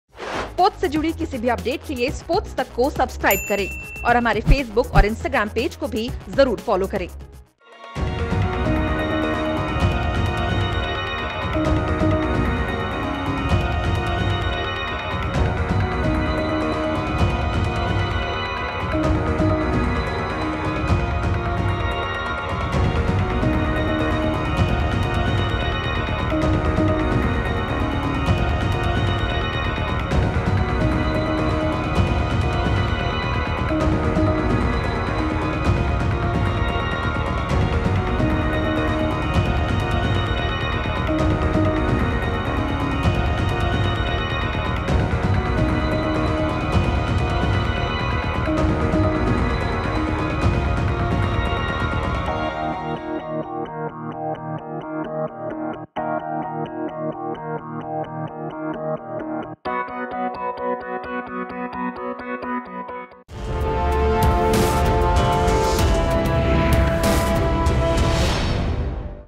ENGLISH PREMIER LEAGUE पर CORONA का कहर, 3 Clubs से मिले 6 मरीज | Sports Tak
English Premier League's first wave of mass coronavirus testing of players and staff found only six people infected at three of the 20 clubs.